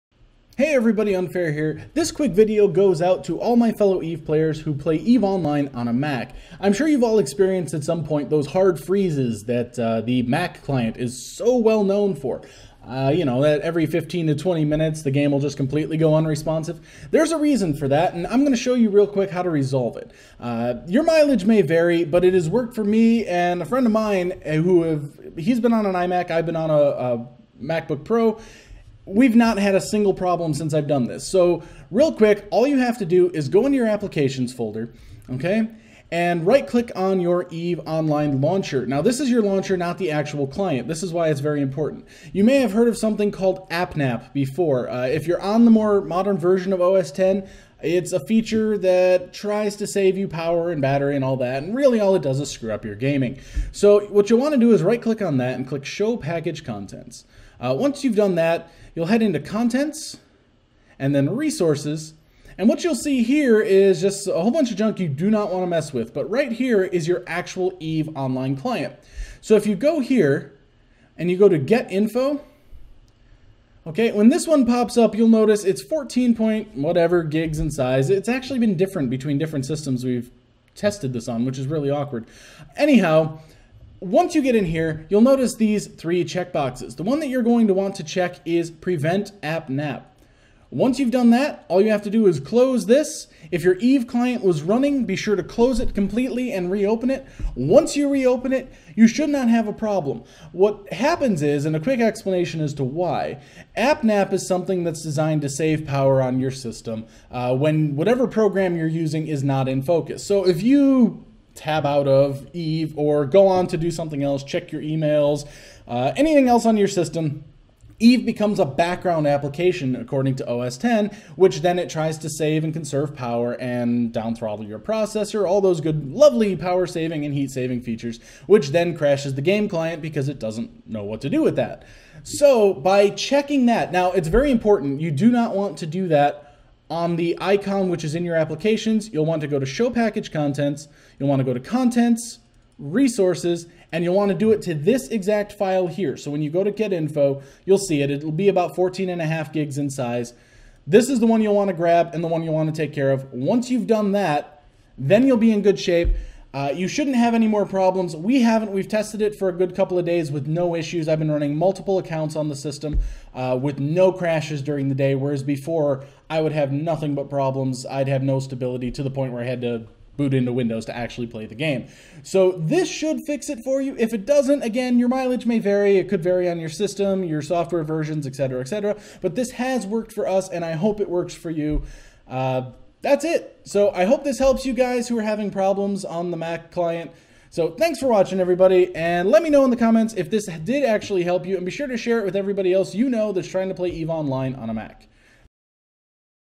EVE Online - Mac Client Crashing / Freezing Solution
A quick video guide to help EVE Online players resolve the stability issues with their Mac OS X client!

Instructions:
- Make Sure EVE Online Is Not Running
- Open Your Applications Folder
- Right Click Your EVE Online Icon And Choose "Show Package Contents"
- Navigate To Contents Then Resources
- Right Click On EVE Online Client
- Select Get Info
- Make Sure "App Nap" Is Checked
- Close Windows, Relaunch EVE Online